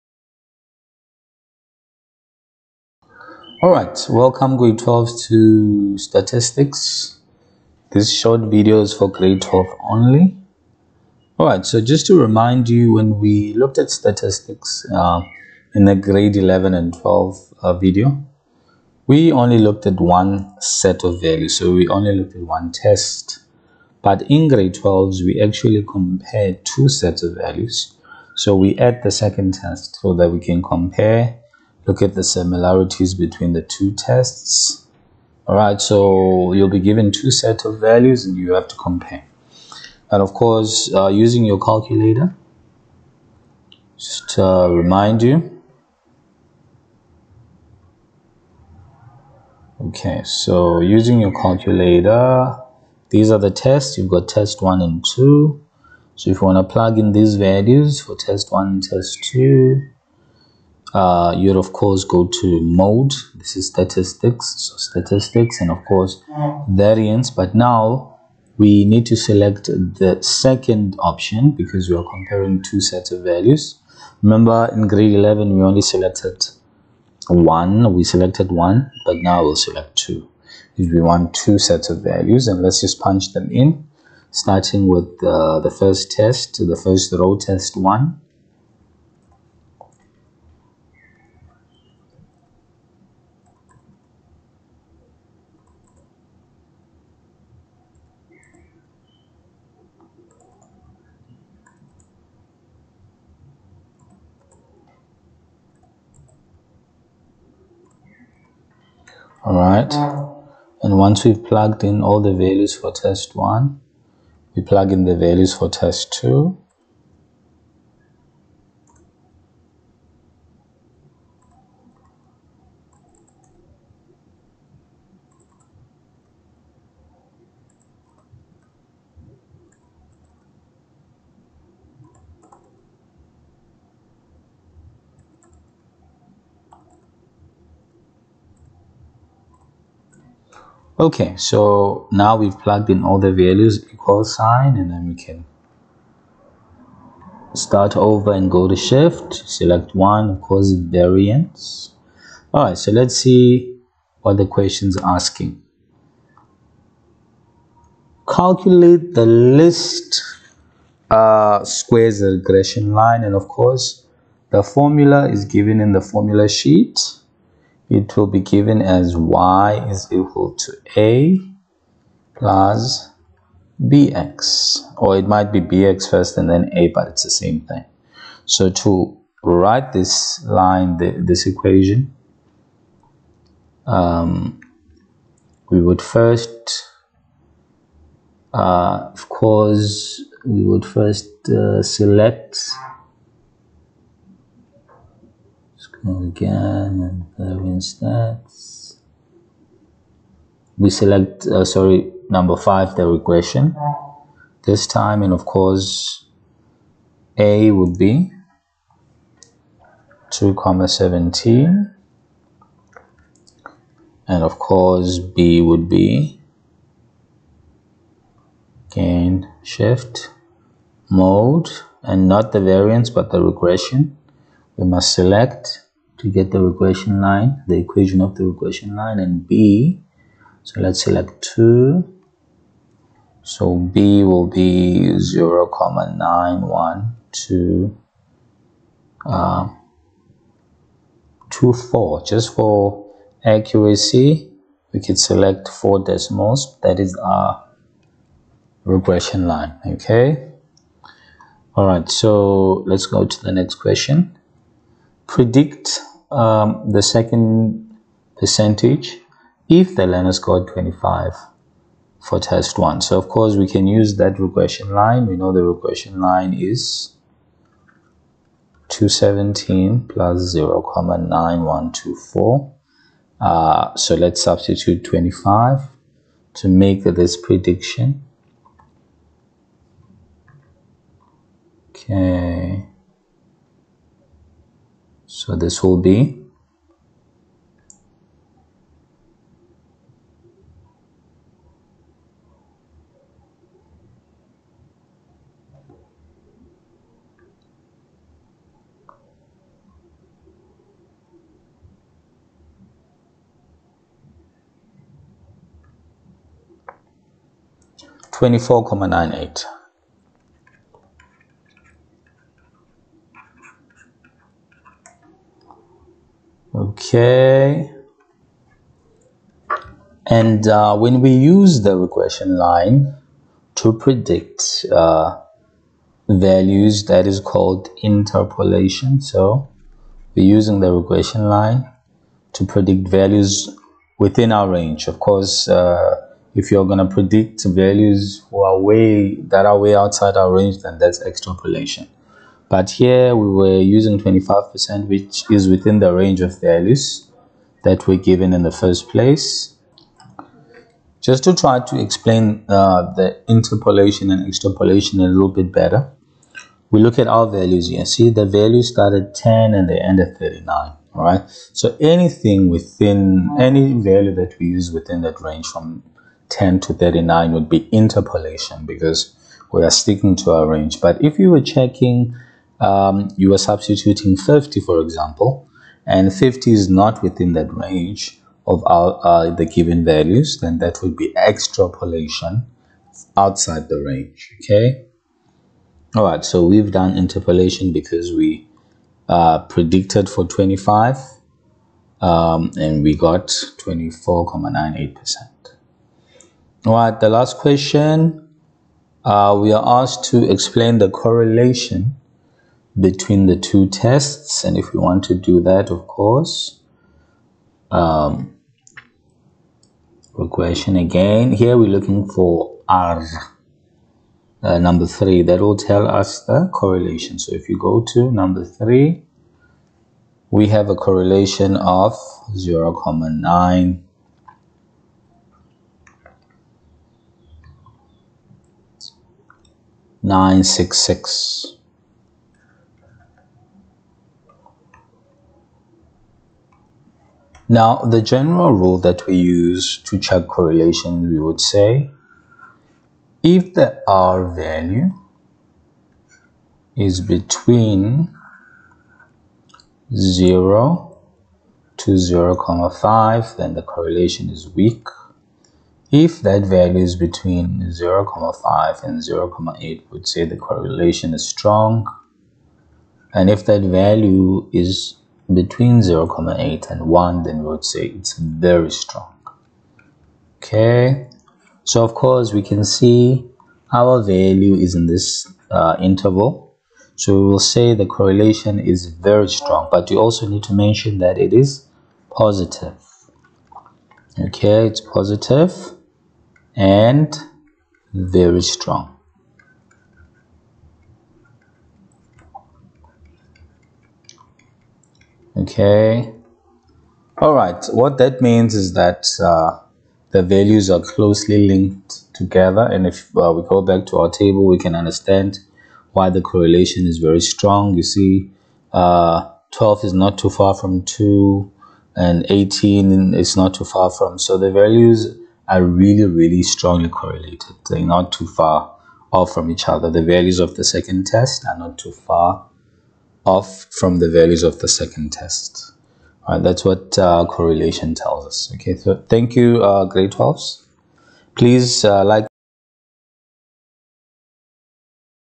Grade 12 Mathematics: Statistics Using a Casio calculator
Grade 12 Maths statistics

Using a Casio calculator for bi-variate data

Finding the least squares regression line

Interpolation